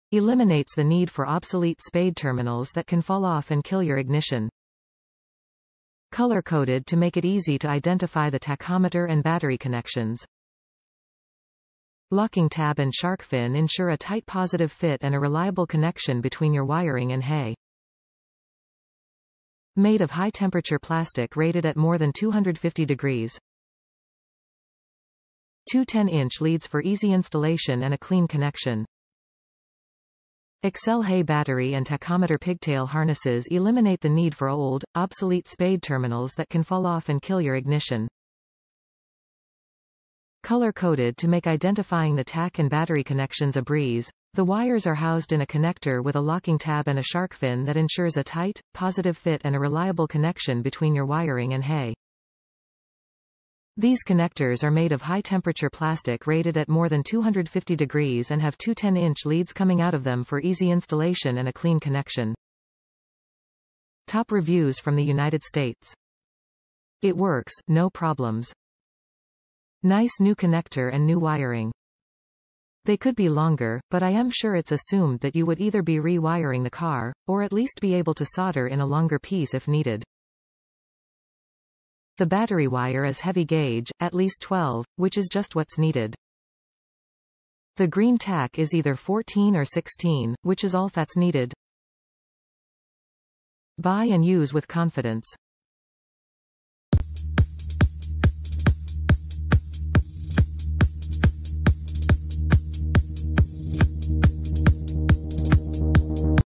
ACCEL 170072 HEI Battery and Tachometer Pigtail Harness, Grey
Eliminates the need for obsolete spade terminals that can fall off and kill your ignition
Color coded to make it easy to identify the tachometer and battery connections
Locking tab and shark fin ensure a tight positive fit and a reliable connection between your wiring and HEI
Made of high temperature plastic rated at more than 250 degrees
Two 10 inch leads for easy installation and a clean connection
ACCEL HEI Battery and Tachometer Pigtail Harnesses eliminate the need for old, obsolete spade terminals that can fall off and kill your ignition. Color-coded to make identifying the tach and battery connections a breeze, the wires are housed in a connector with a locking tab and a shark fin that ensures a tight, positive fit and a reliable connection between your wiring and HEI. These connectors are made of high-temperature plastic rated at more than 250 degrees and have two 10-inch leads coming out of them for easy installation and a clean connection.

it works, no problems
Nice new connector and new wiring. They could be longer, but I'm sure its assumed that you would either be re-wiring the car, or at least be able to solder in a longer piece if needed. The battery wire is heavy gauge, at least 12, which is just what's needed. The green tach is either 14 or 16, which is all that's needed. Buy and use with confidence.